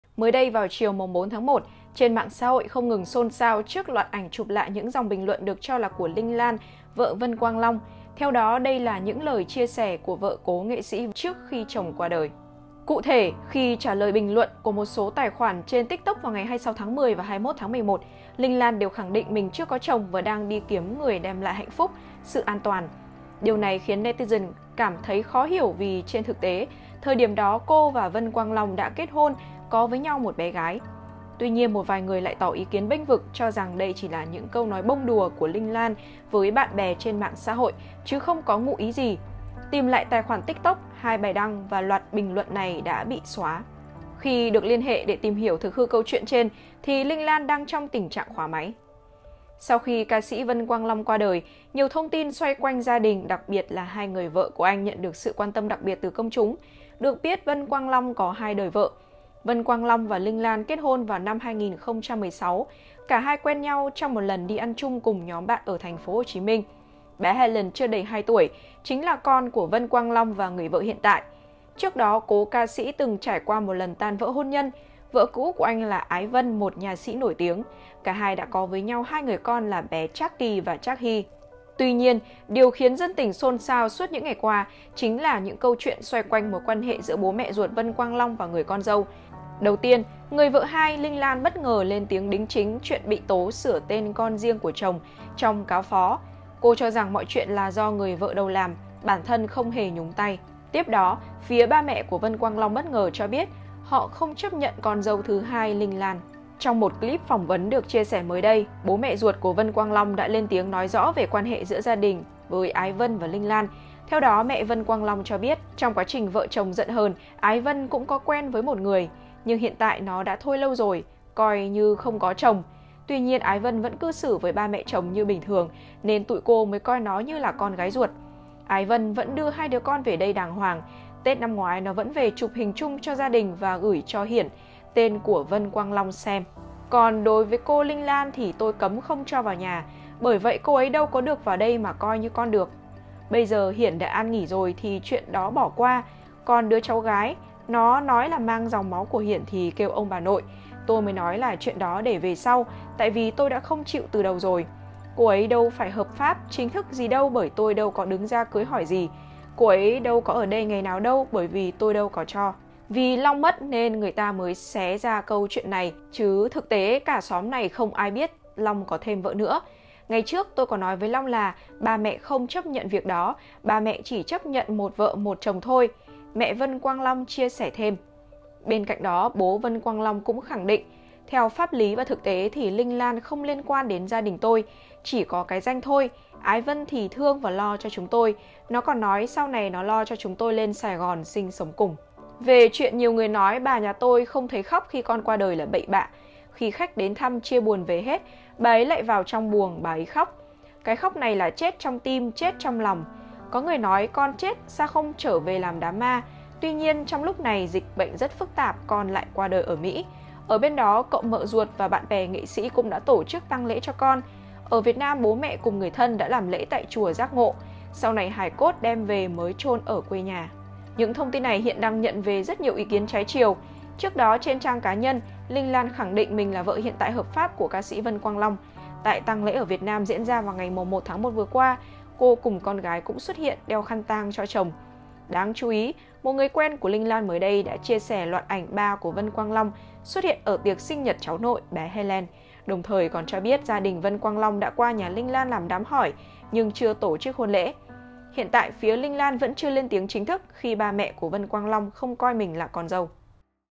Linh Lan khẳng định chưa chồng | Thực hư câu chuyện chưa được Vân Quang long cưới hỏi?
Mới đây vào chiều ngày 4/1, trên mạng xã hội không ngừng xôn xao trước loạt ảnh chụp lại những dòng bình luận được cho là của Linh Lan - vợ Vân Quang Long. Theo đó, đây là những lời chia sẻ của vợ cố nghệ sĩ Vân Long trước khi chồng qua đời.